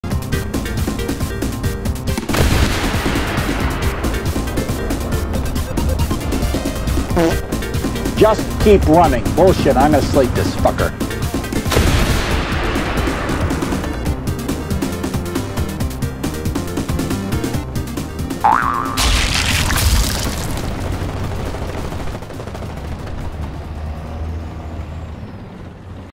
He tried to parry the missile
And I couldn't even see him!

Featured vehicles:
Marder A1-
IS6